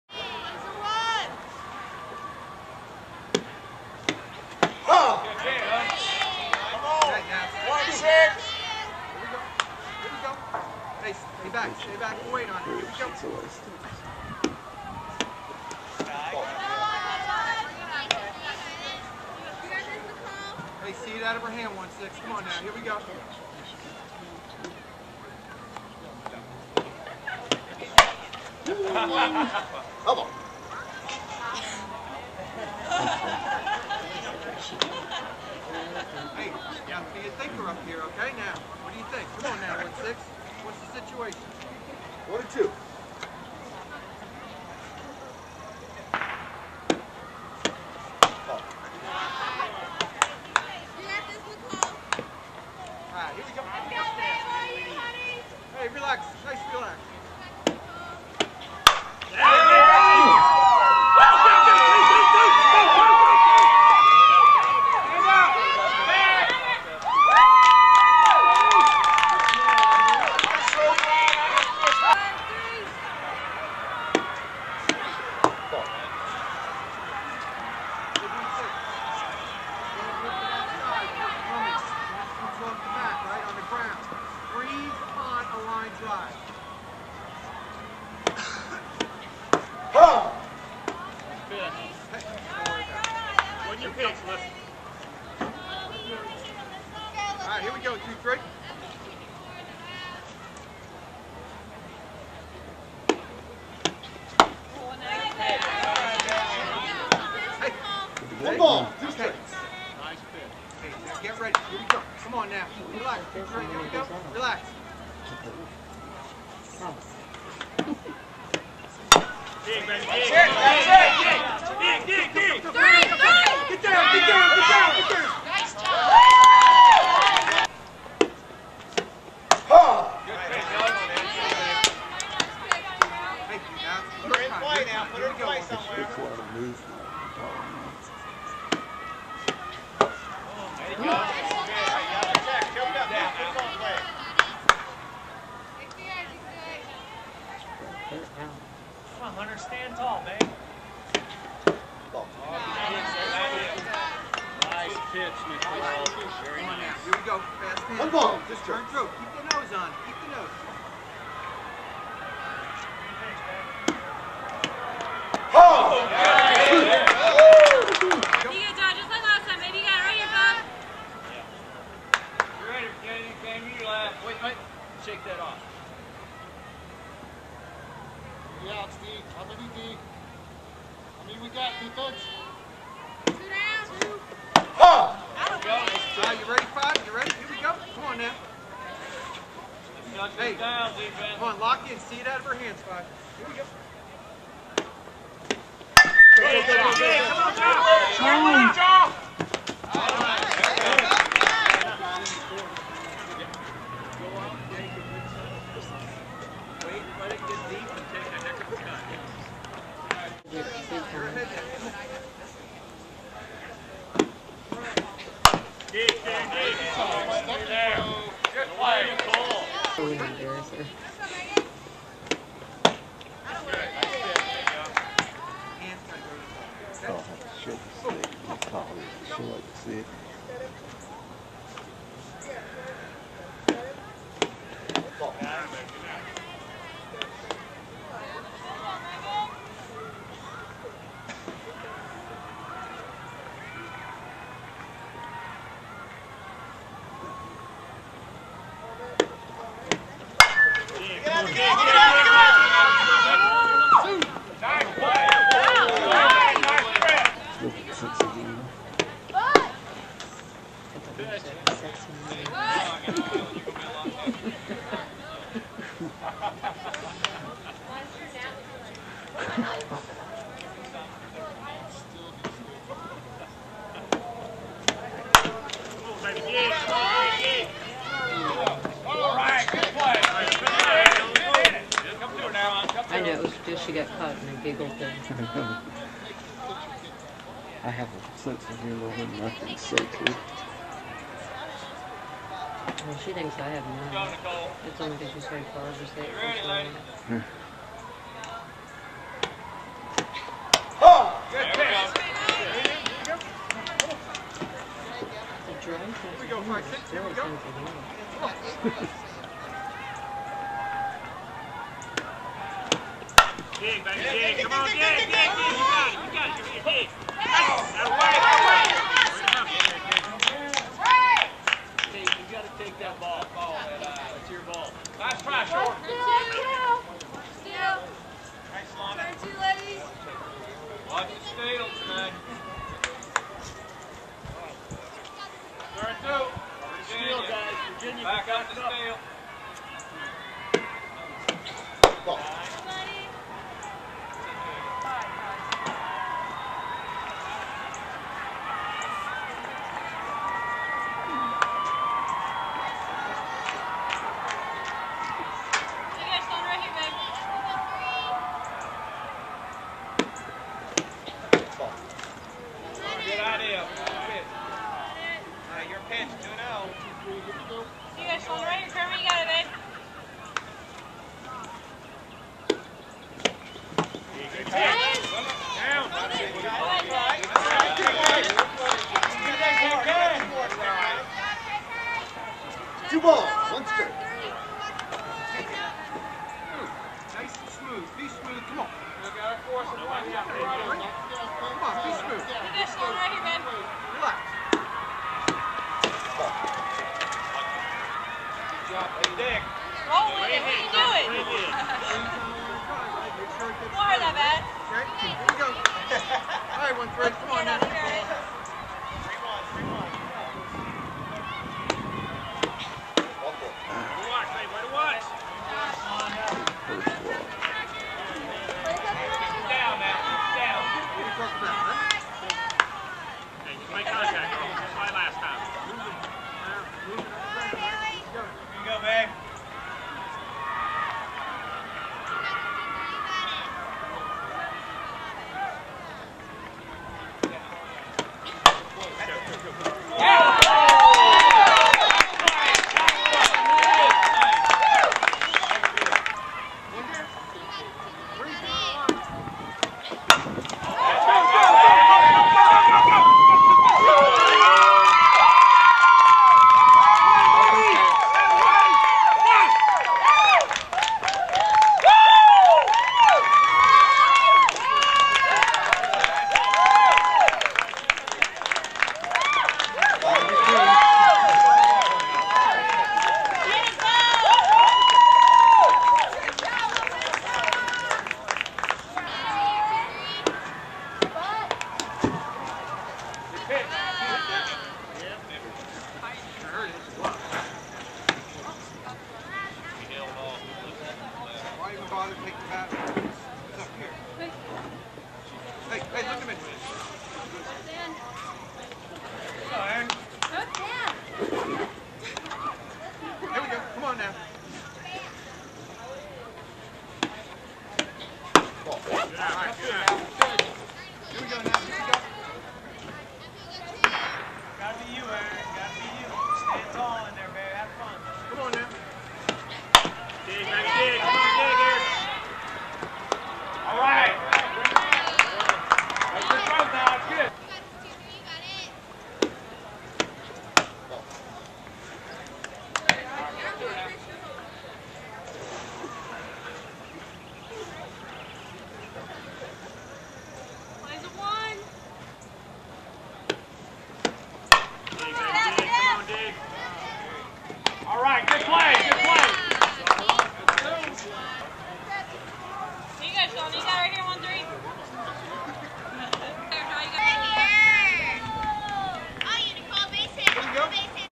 Nicole Gilkeson - 03/10/2012
Nicole pitching for the Roswell Orange Crush 14u, playing against the East Cobb Edge 16u.